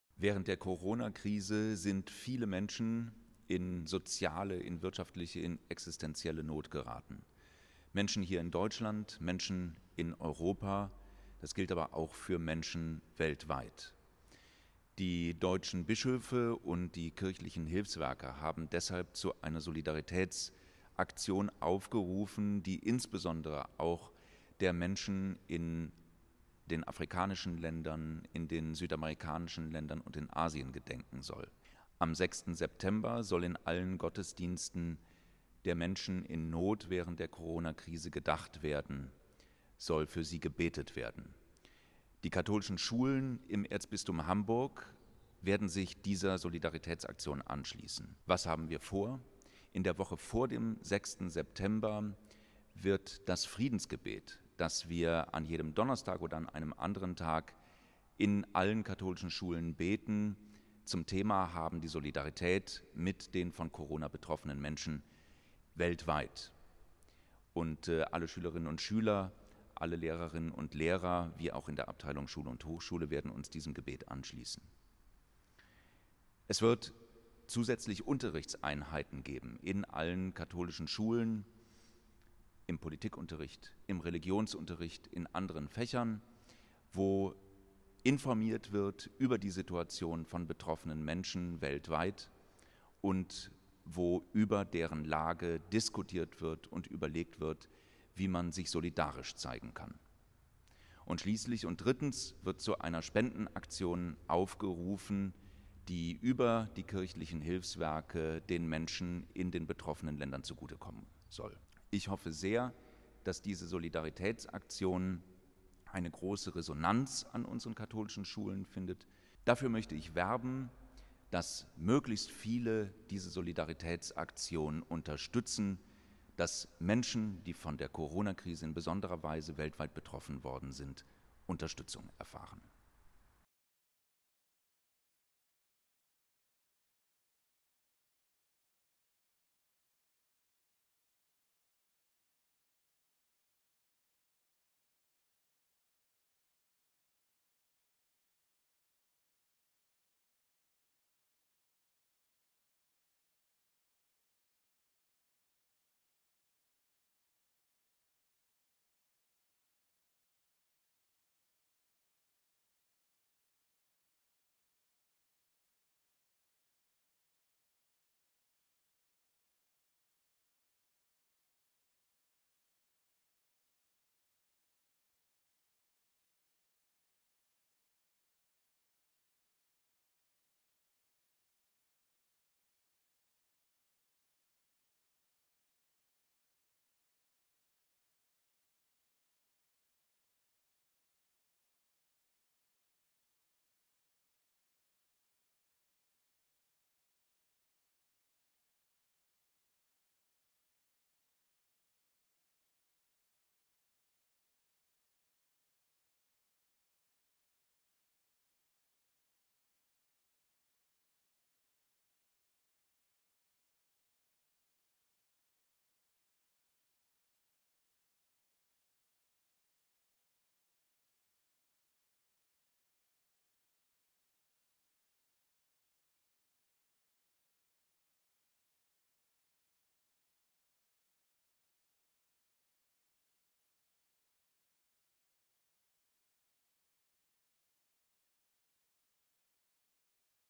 Hamburgs katholische Schulen zeigen Solidarität mit den Leidtragenden der Corona-Pandemie
Hamburgs katholische Schulen zeigen Solidarität mit den Leidtragenden
der Corona-Pandemie

Die katholischen und evangelischen Hilfswerke rufen angesichts der Corona-Krise und der existentiellen Notlage, in die viele Menschen insbesondere in Afrika, Lateinamerika und Asien geraten sind, zu einer besonderen Gebets- und Spendenaktion auf.  Am Sonntag, den 6. September wird in allen Gottesdiensten für die Menschen in diesen Ländern gebetet und es wird eine Sonderkollekte gehalten. Hamburgs katholische Schulen werden sich an dieser Aktion beteiligen und das Thema in der Woche vor dem Solidaritätssonntag – also vom 31.08. bis 04.09. – aufgreifen, u.a. mit Gebeten, inhaltlichen Impulsen und in der konkreten Unterstützung von Hilfsprojekten.

Wer eine Spende überweisen möchte, der nutze bis zum 10.9. folgendes Aktions-Konto des Erzbistums Hamburg bei der Darlehnskasse Münster: IBAN: DE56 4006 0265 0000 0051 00.
Die Gesamtsumme wird dann zeitnah auf das Konto der Deutschen Bischofskonferenz weitergeleitet.